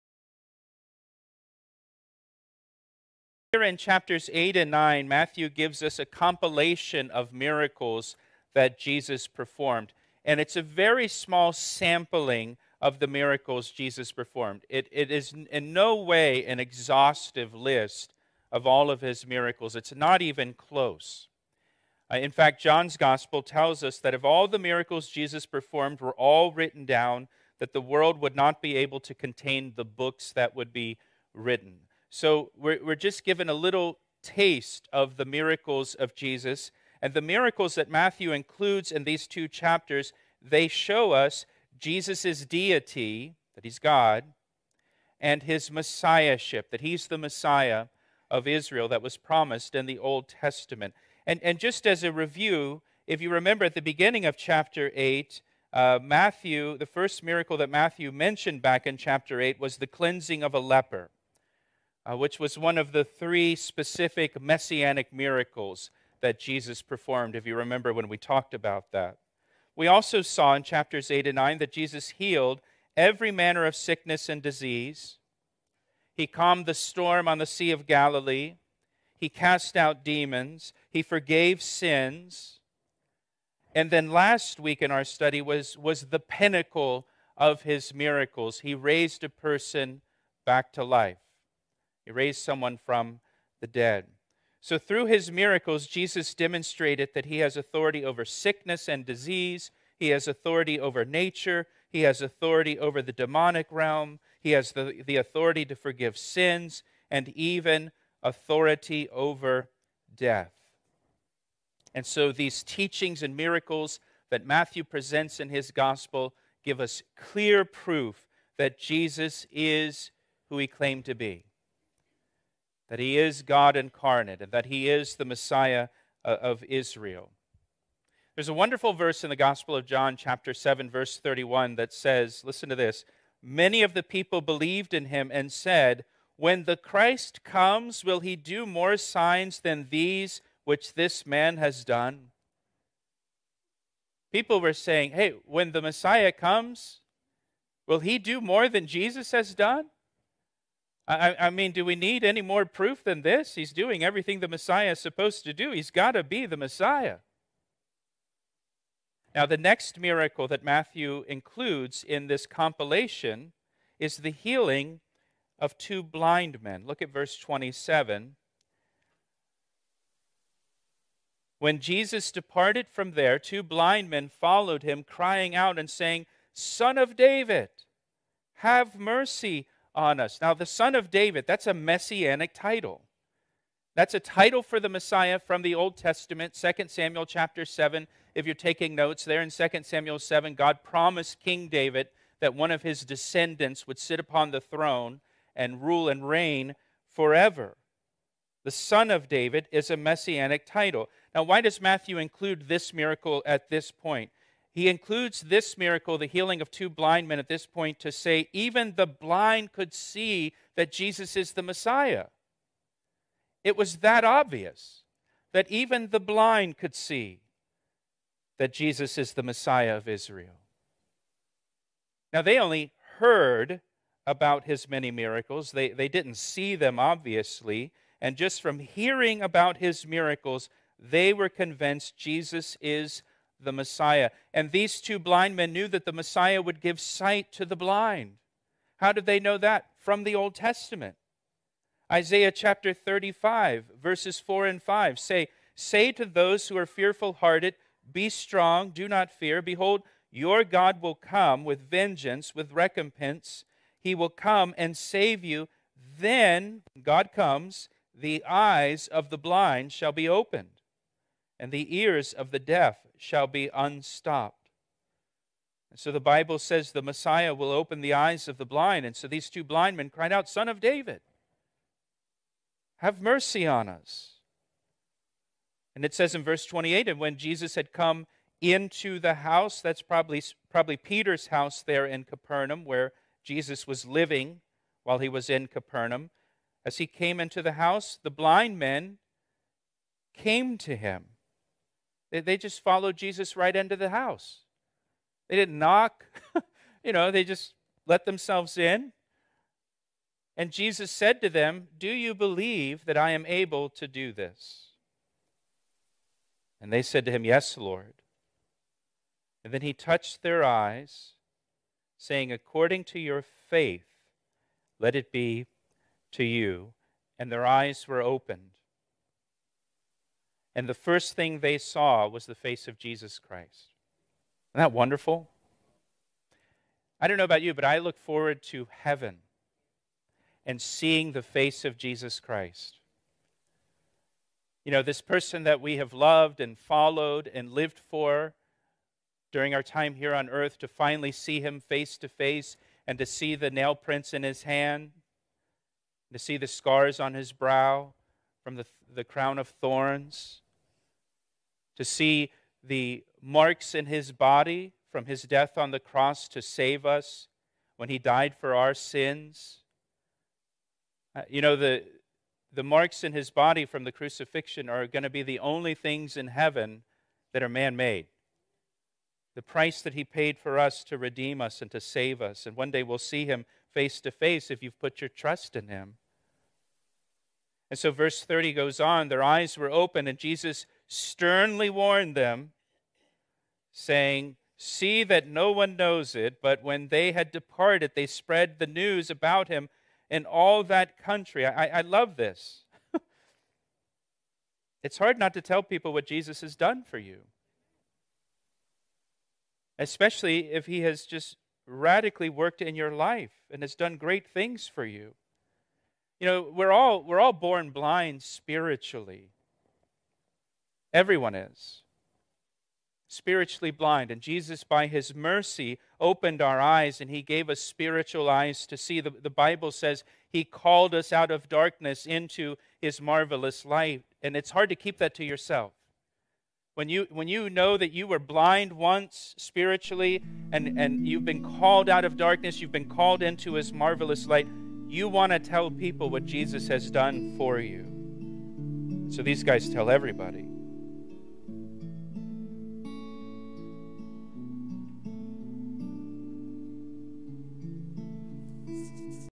Even The Blind See He's Messiah - Matthew 9:27-31
[Mat 9:27-31 NKJV] 27 When Jesus departed from there, two blind men followed Him, crying out and saying, "Son of David, have mercy on us!" 28 And when He had come into the house, the blind men came to Him. And Jesus said to them, "Do you believe that I am able to do this?" They said to Him, "Yes, Lord." 29 Then He touched their eyes, saying, "According to your faith let it be to you." 30 And their eyes were opened. And Jesus sternly warned them, saying, "See [that] no one knows [it]." 31 But when they had departed, they spread the news about Him in all that country.